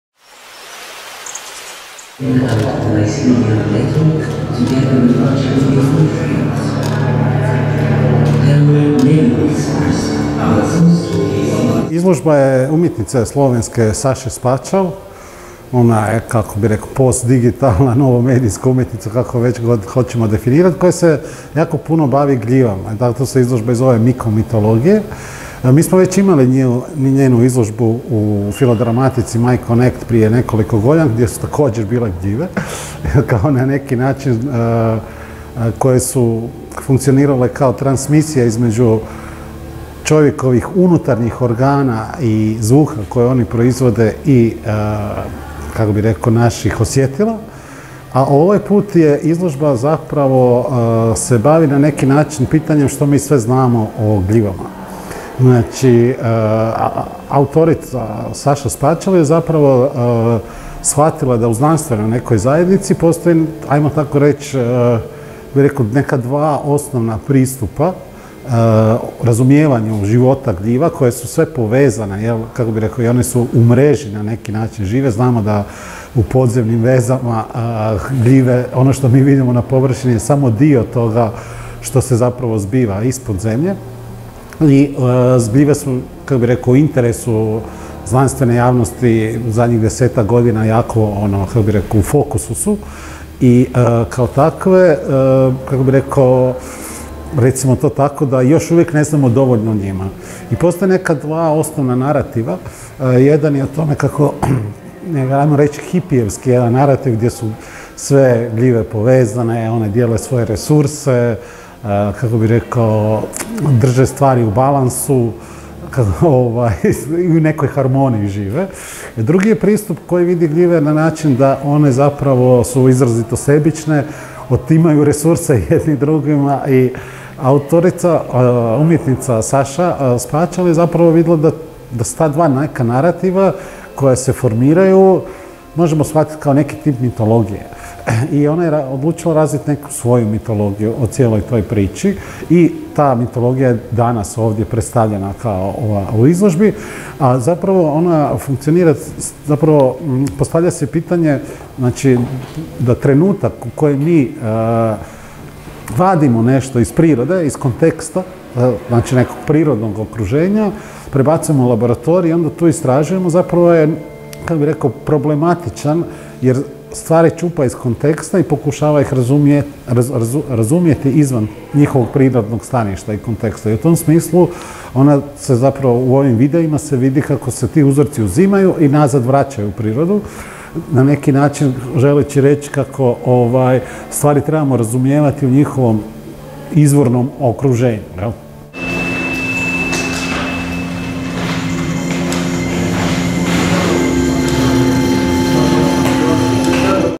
MojaRijeka.hr - U Filodrammatici otvorena izložba MikoMitologije: mit o raskidu
U galeriji Filodrammatice otvorena je izložba "MikoMitologije: Mit o Raskidu" slovenske novomedijske umjetnice" Saša Spačal, a riječ je o zanimljvoj biotehnološkoj umjetničkoj instalaciji koja tematizira veze između gljiva.

Autorica videa je Sara Blažić.